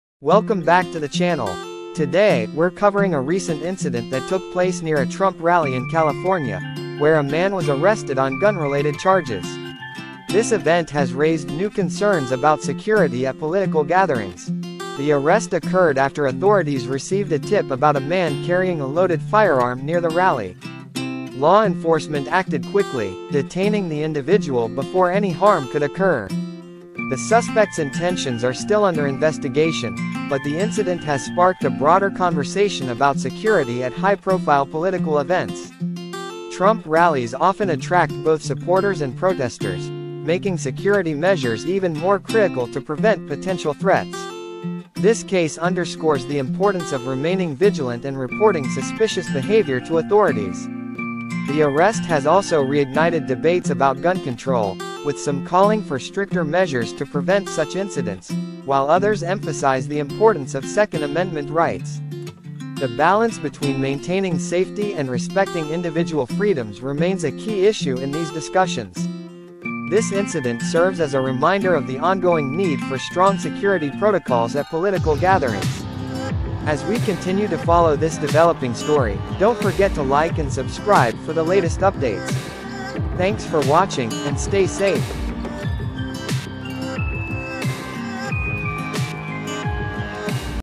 Man Arrested Near Trump Rally on Gun Charges Sparks New Security Concerns
A man was arrested with a loaded firearm near a Trump rally in California. Could this be a wake-up call for tighter security at political events?
A man was arrested near a Trump rally in California on gun-related charges, raising concerns about security at political events and sparking debates over gun control. Learn more about the incident and its implications.